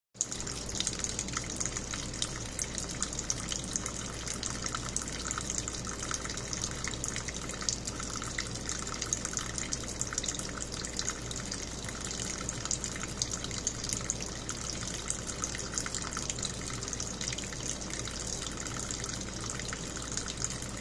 Kitty Water Fountain
Pumpkin would rather drink from a faucet